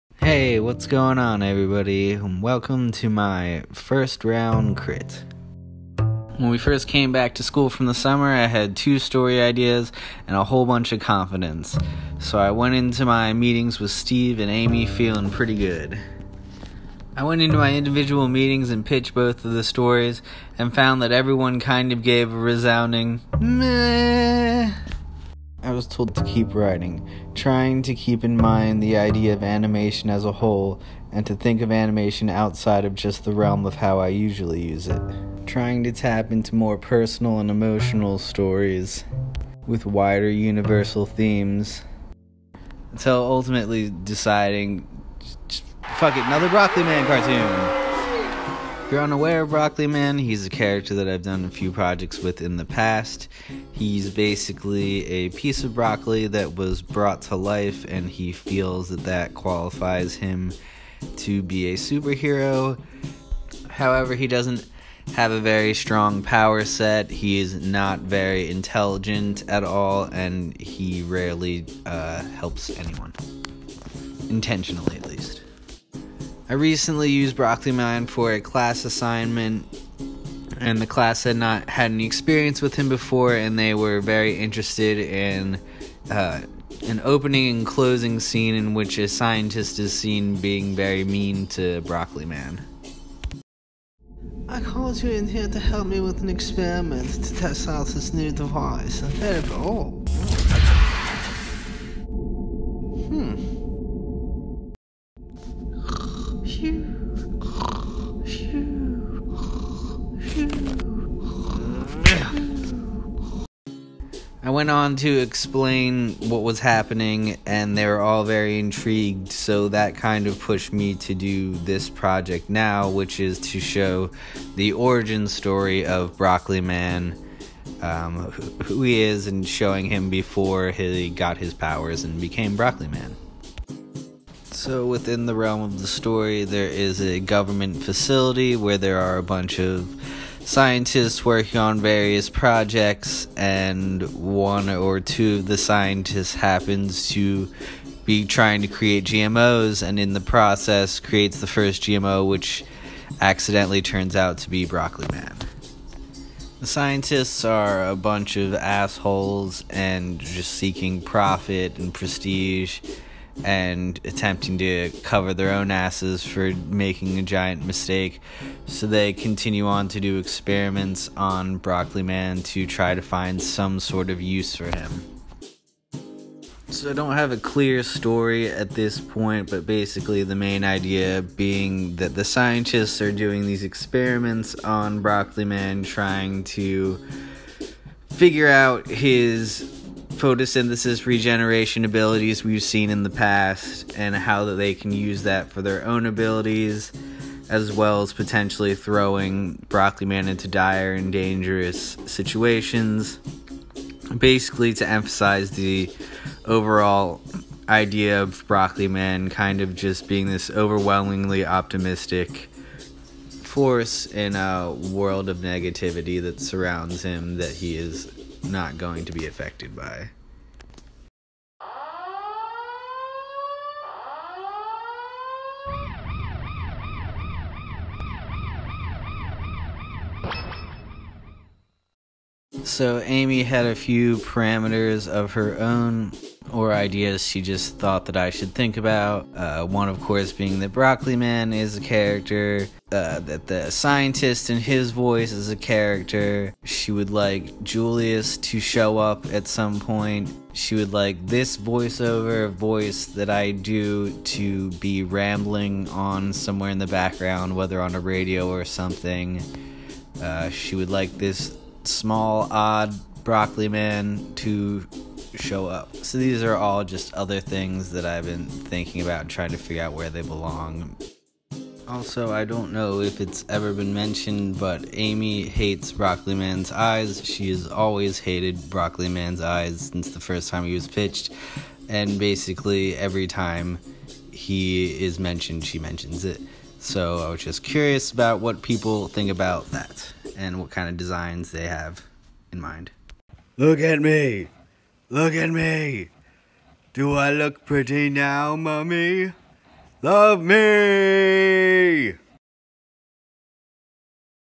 Degree Project First Pitch/...kind of
Here’s a video for my class. I talk about a project I would like to work on, and ask for advice.